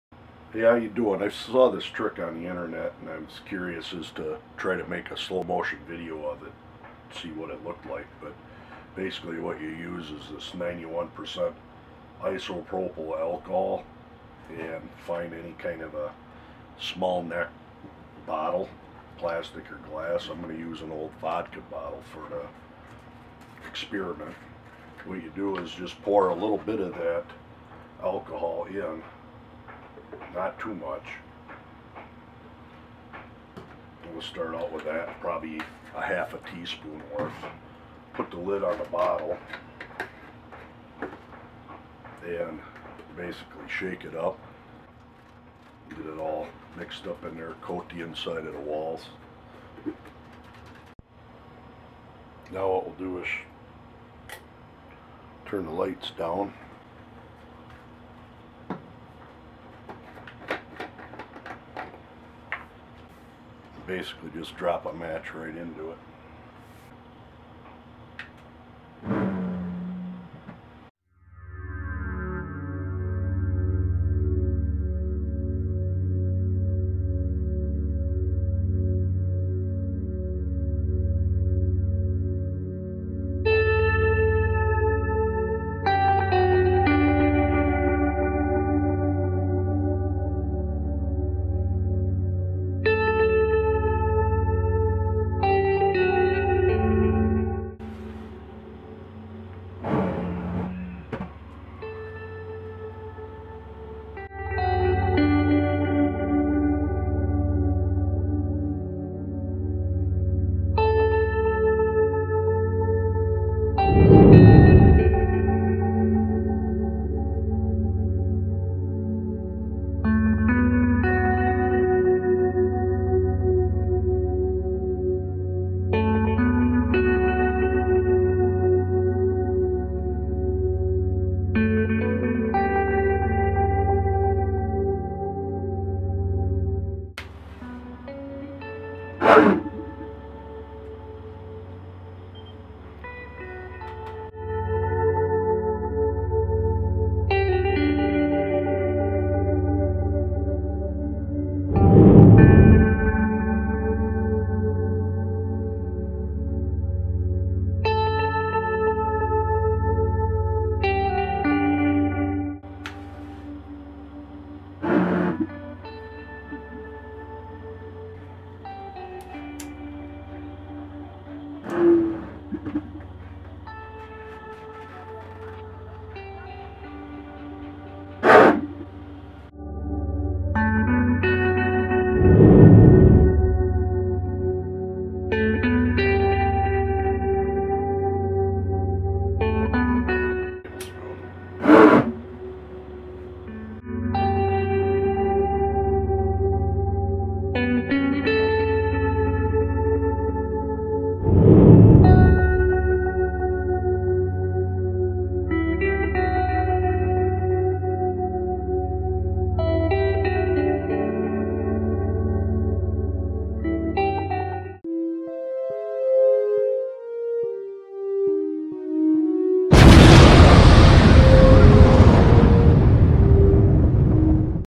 Isopropyl Alcohol Flame Cannon in Slow Motion
Alcohol fumes igniting in a bottle in Slow Motion
Filmed with a Nikon D5100 and Prime Lens
Slow Motion from a Casio EX-F1
Do not try this at home. All precautions were taken and tested before filming